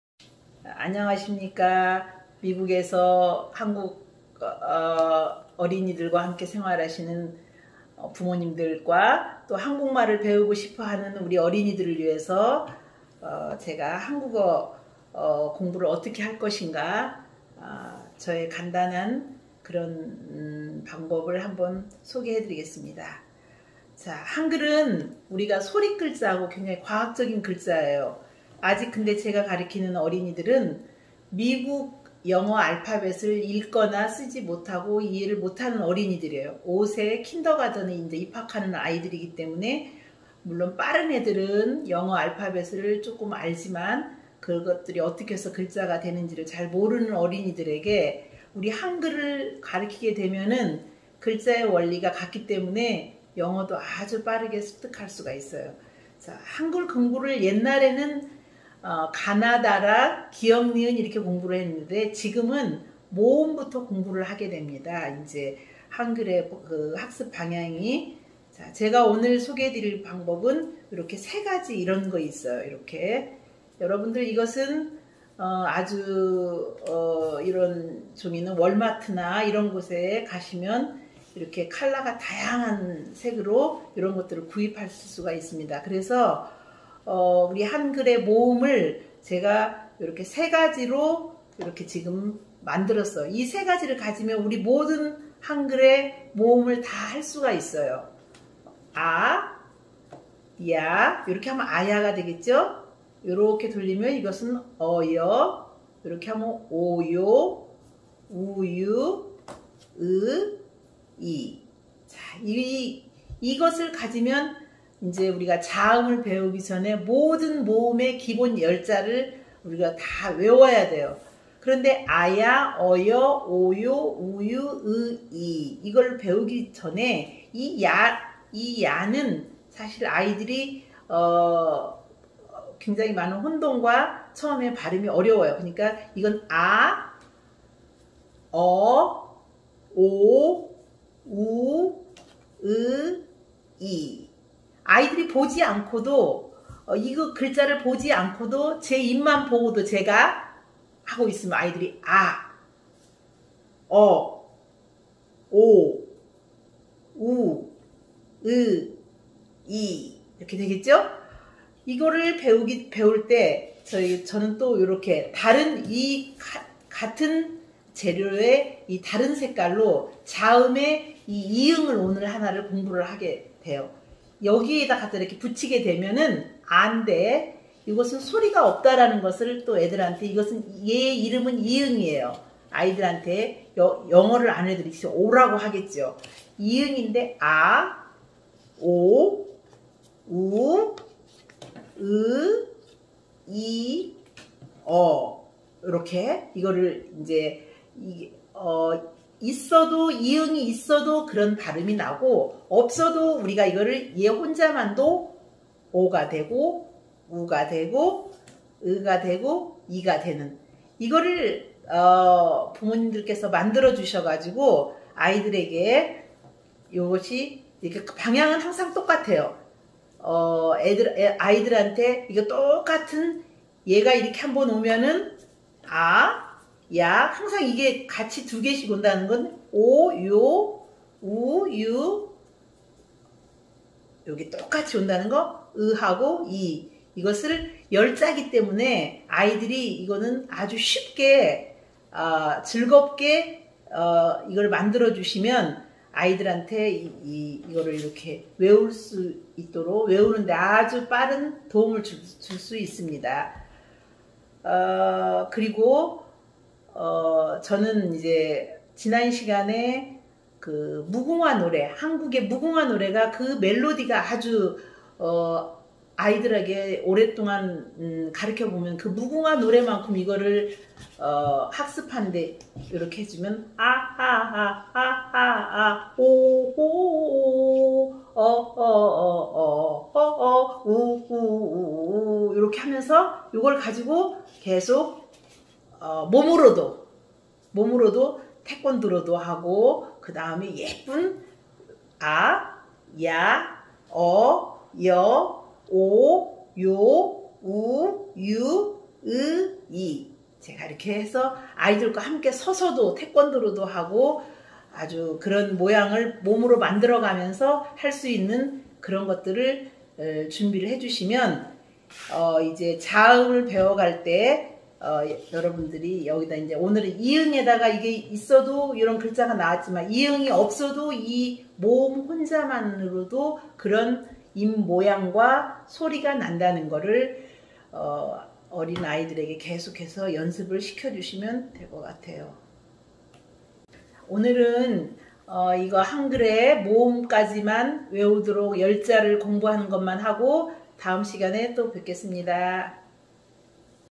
처음 한글은 모음부터 - The first Hangul is from the vowels.
애틀랜타 한국학교에서 14년째 아이들을 가르치고 있는 한글정쌤 입니다.

애틀랜타 한국학교에서 5세 아이들을 25년째 가르치고 있는 박운혜 선생님과 함께 해 봅니다. 미국에서 자라는 아이들은 집에서는 간단한 한국어로 부모님과 이야기하고 학교에서는 영어를 사용하기 때문에 처음 한글을 배우기가 쉽지 않습니다.
아직 영어도 모르는 아이들에게 어떻게 하면 쉽게 한글을 가르칠수 있는지 맨 처음 과정인 모음부터 공부해 보겠습니다.